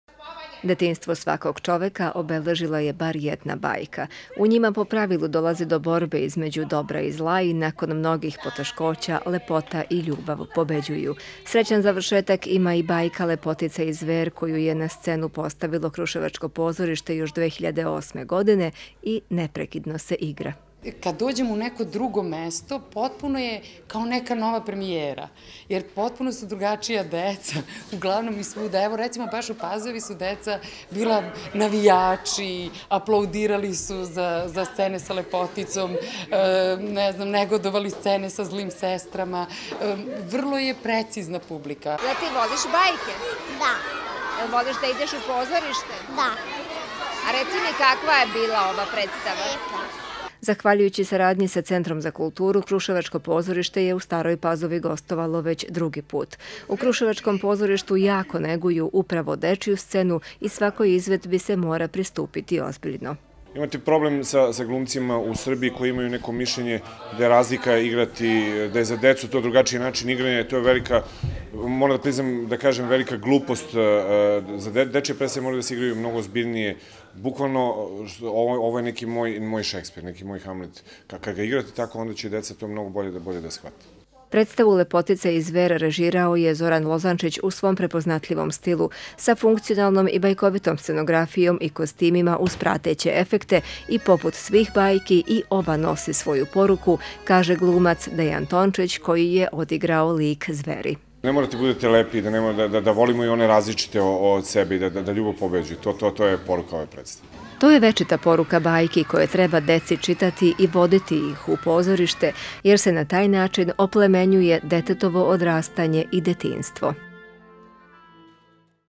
Predstava "Lepotica i zver"
Najmlađa staropazovačka publika je ponovo mogla da uživa u pozorišnoj predstavi Kruševačkog pozorišta. Zahvaljujući saradnji sa Centrom  za kulturu St. Pazova, ovo pozorište, koje neguje dečju scenu, gostovalo je već drugi put u Pazovi. Juče je odigrana bajka Lepotica i zver.